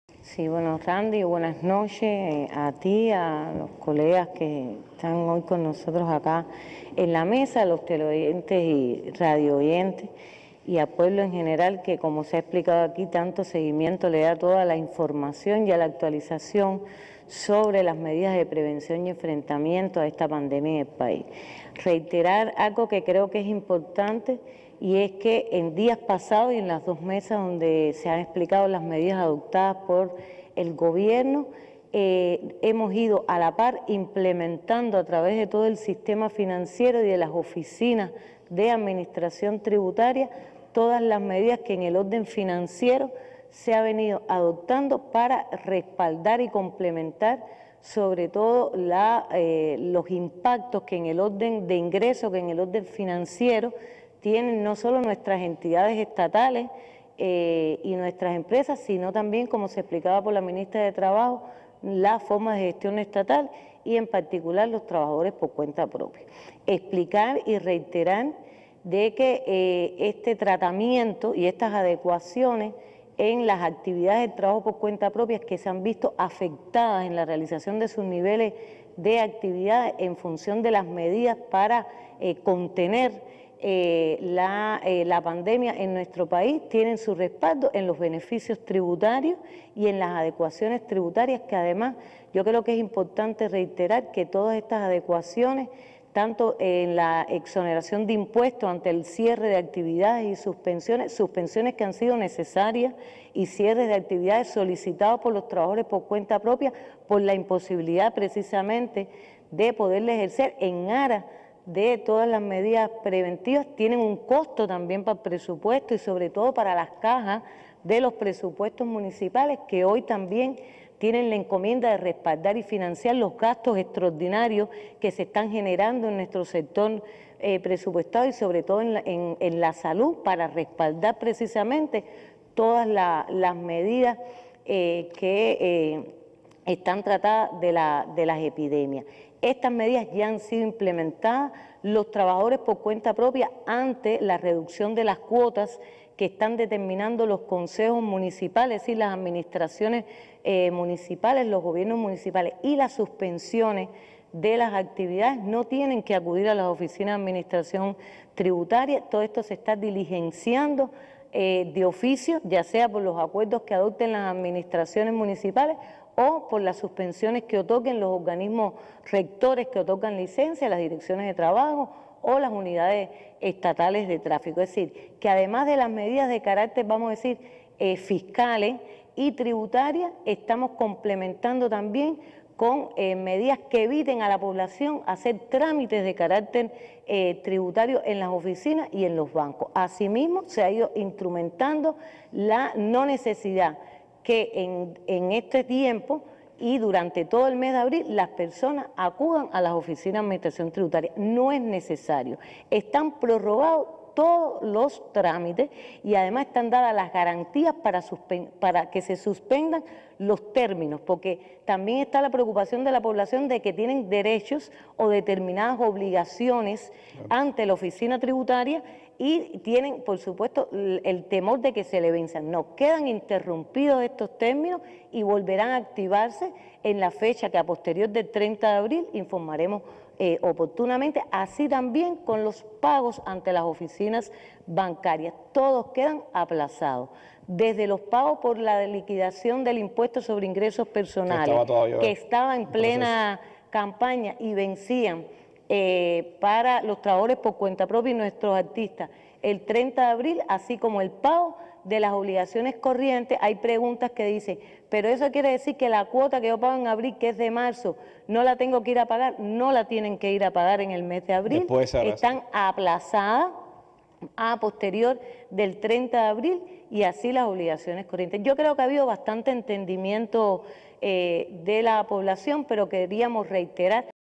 Cuba frente a la COVID-19:Ministerio de Finanzas y Precios
@MeisiBWeiss por Ministerio de Finanzas y Precios
Hemos recibido quejas sobre todo en LaHabana por sobreprecios en productos necesarios, esto hay que enfrentarlo de manera rápida y con herramientas que nos permita detectarlos, se evalúan nuevas normativas para aplicar multas más rigurosas.